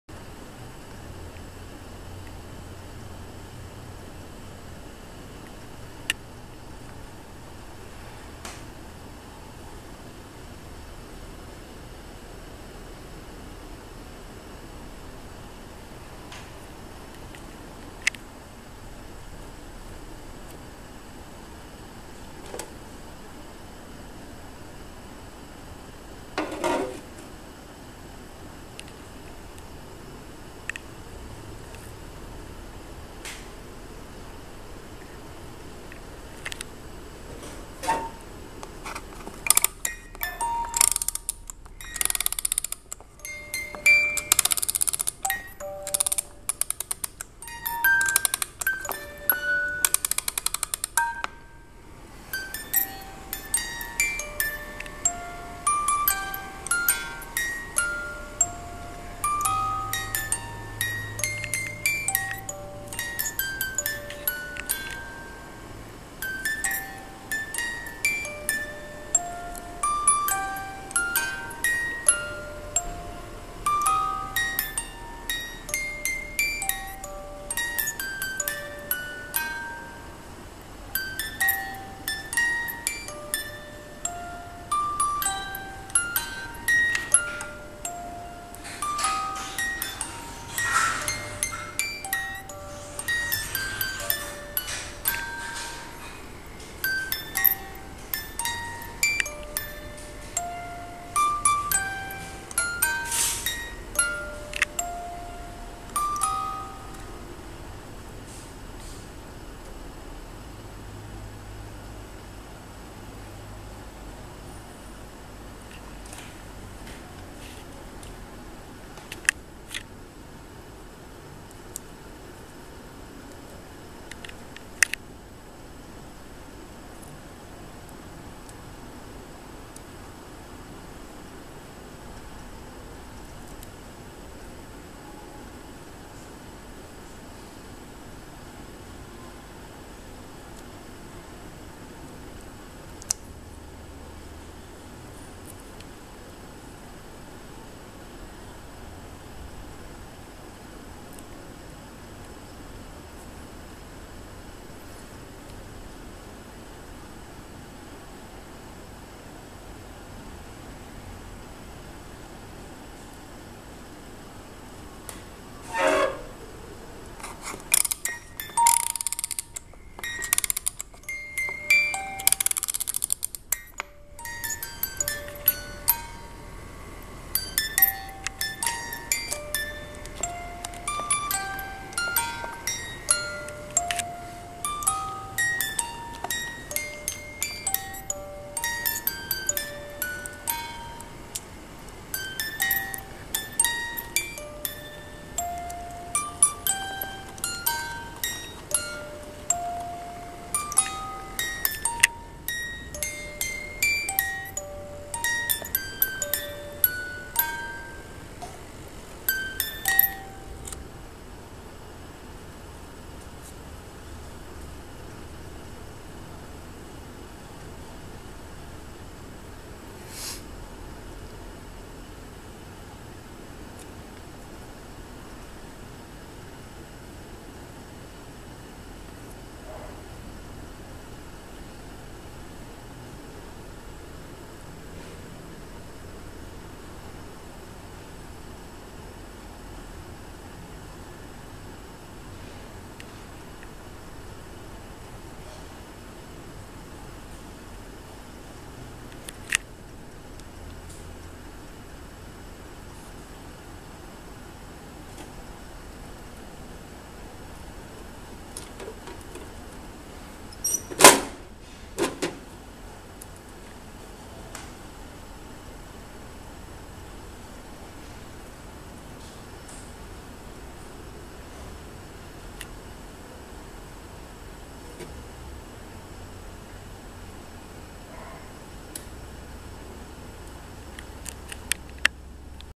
DIY Miniature House - The Wedding Room
My DIY miniature Wedding room for my best friend as her wedding gift , Bigger DIY miniature home stay tune ~